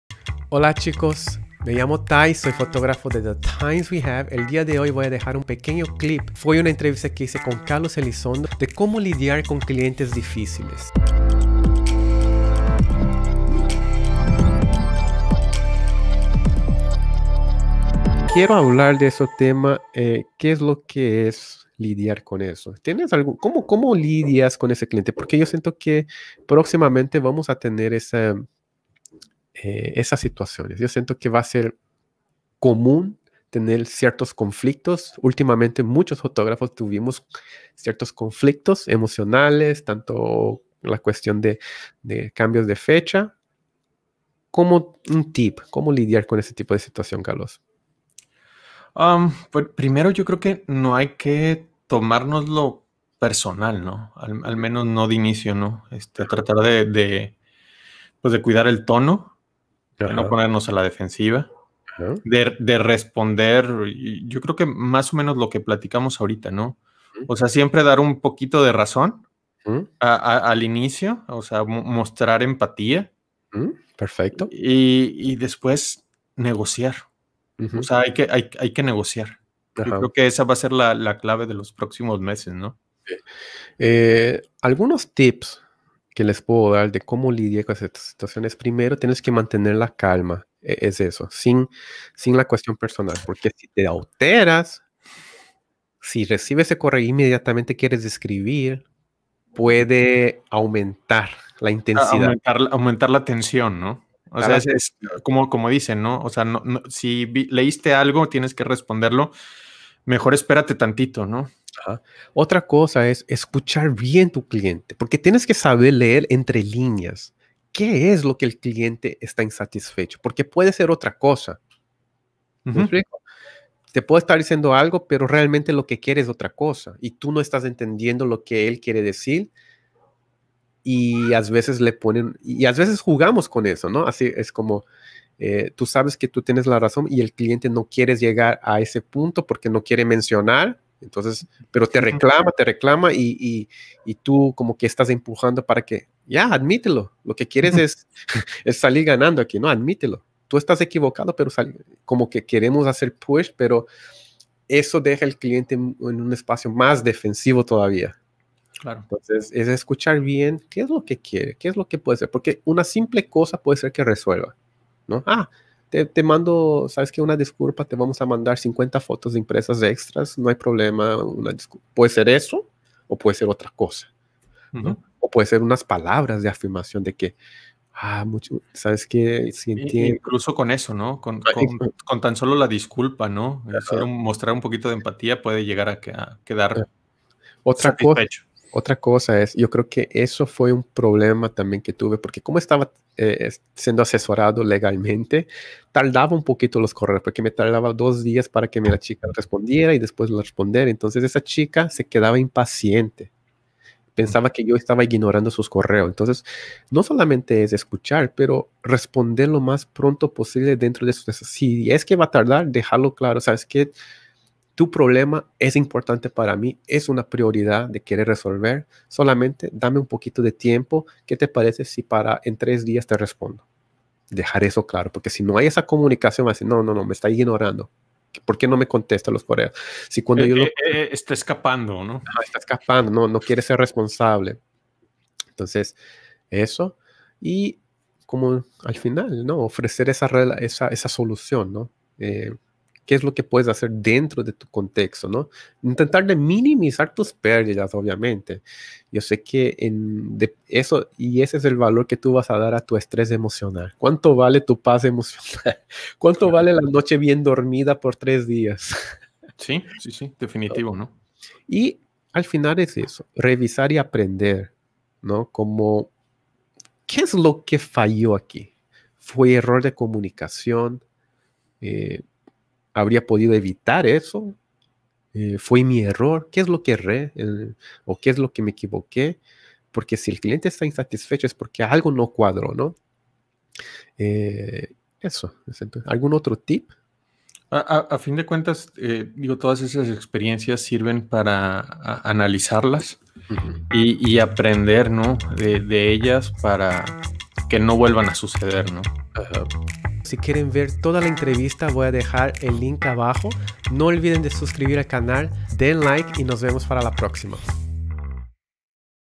¿Cómo lidiar con un cliente difícil en la fotografía de boda?
Una parte de la entrevista donde hablamos de ¿Cómo lidiar con cliente difíciles?

Sigan nuestras páginas y les invito que sea parte del grupo Be Here Project.